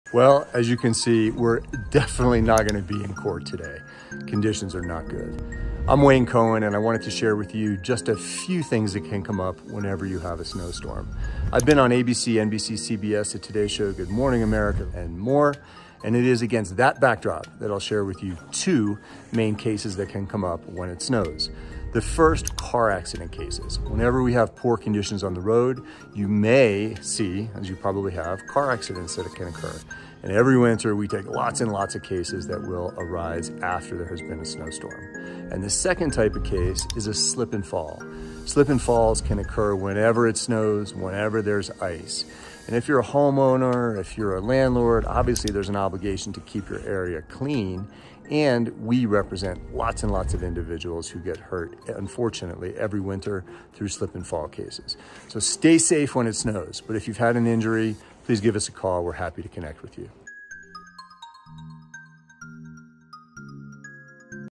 The Dangers of Snow Storms
Wayne Cohen details the dangers and accidents that tend to occur after a big snow storm.